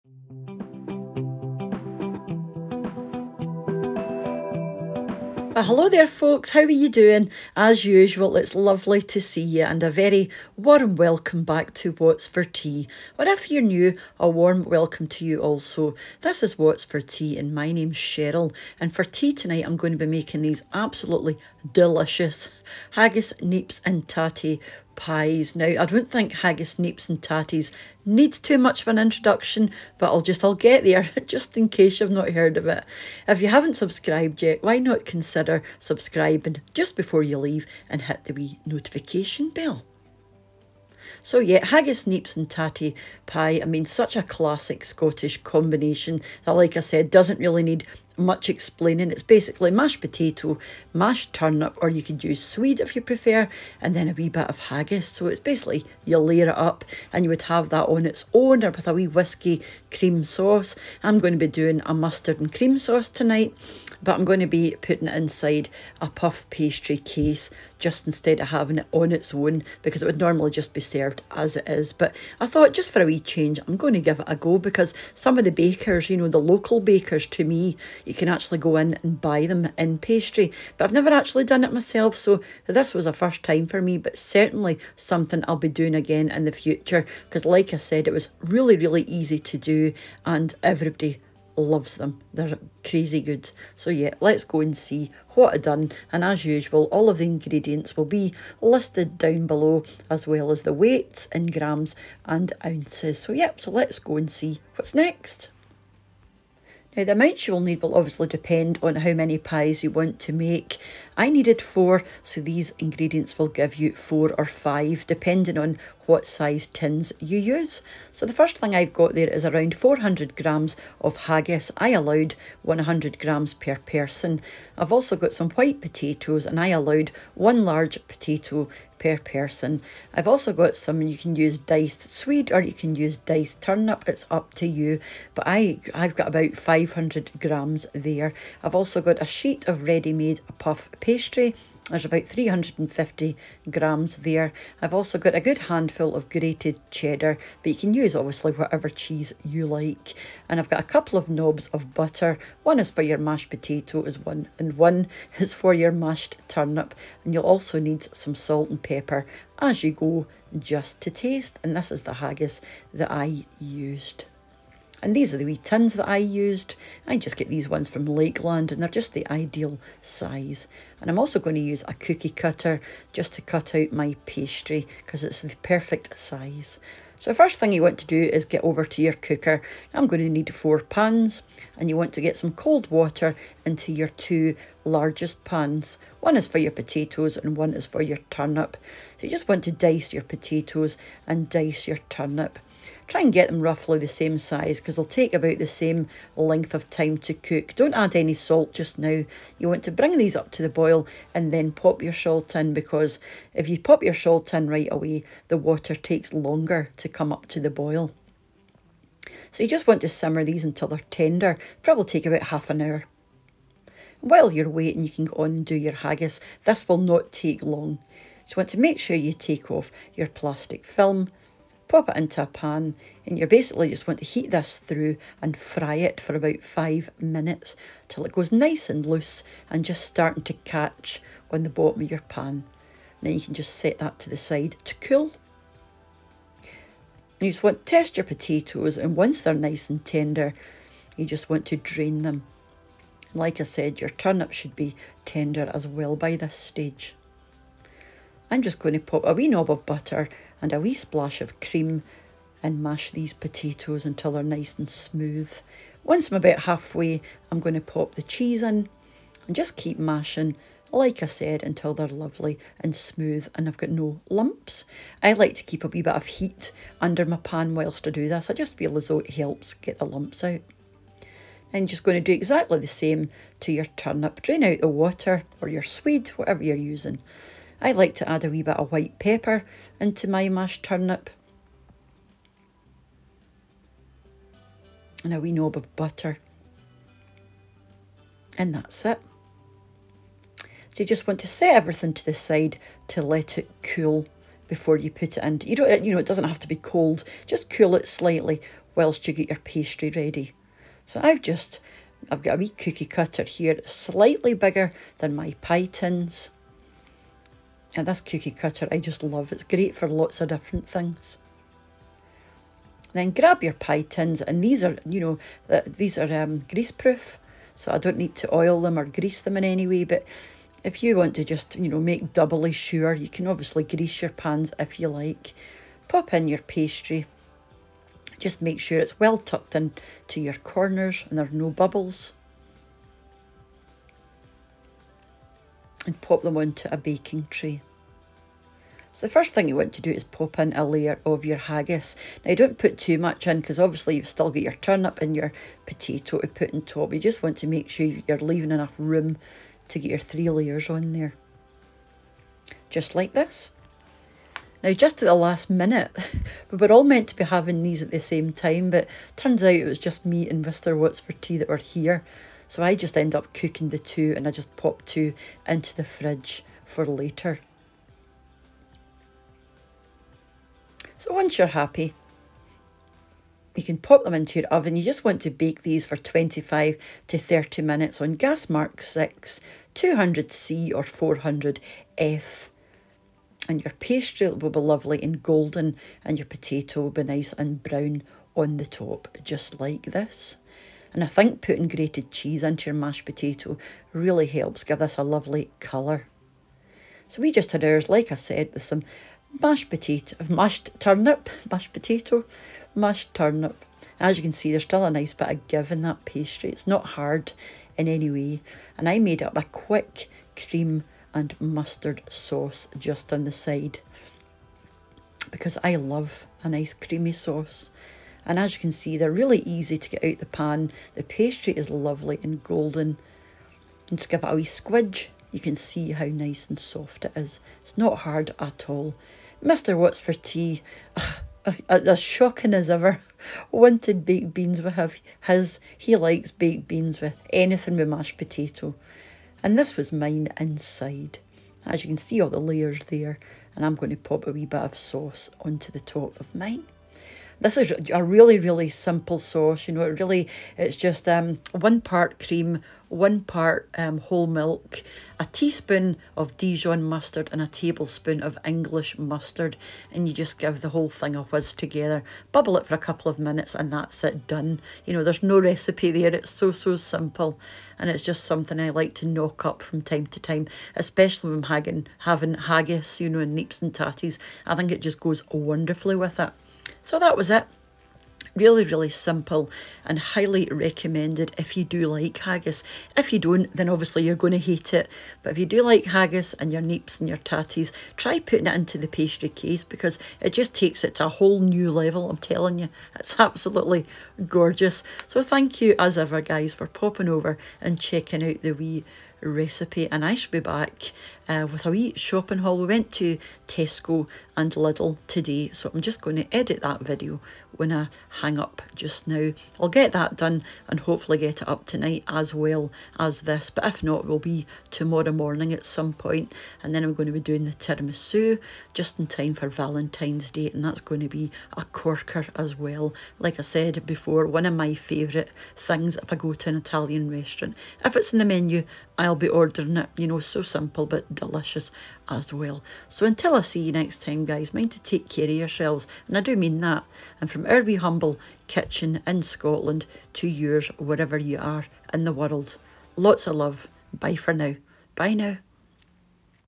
Scottish Haggis, Neeps & Tattie pie :) Recipe | Burns supper
Hello folks!

A very warm welcome back to What’s For Tea :) for tea tonight I’ll be making some delicious haggis, needs & tattle pies :)

Haggis, Neeps (turnip or swede) and tatties (potatoes) is such a classic Scottish dish that it really doesn’t need much explaining, and I’m sure everyone has heard of it! But tonight I’ll be making it into a pie! They turned out wonderful and something I’ll definitely be making again!

Everything I used will be mentioned below incase you want to make them yourself. Thanks for stopping by,

-Cheryl x

Makes 4 pies

Around 400g/14oz (or four slices) Haggis
4 Good sized white potatoes (1 per person)
500g/17oz Turnip or swede
1 Sheet of puff pastry (around 350g/12oz)
A handful of grated cheddar (or cheese of your choice)
A couple of knobs of butter
Salt & white pepper to taste